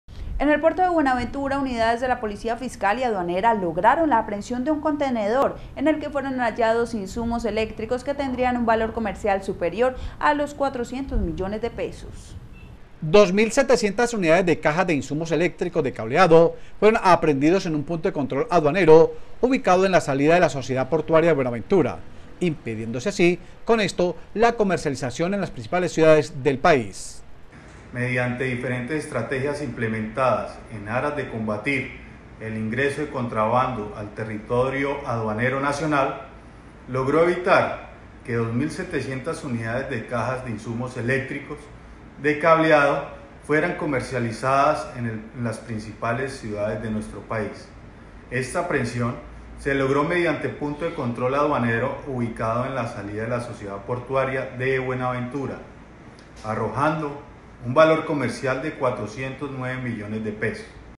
Incuataron cargamento de insumos eléctricos
En el puerto de Buenaventura, unidades de la policía fiscal y aduanera, lograron la aprehensión de un contenedor, en el que fueron hallados insumos eléctricos que tendrían un valor comercial superior a los 400 millones de pesos.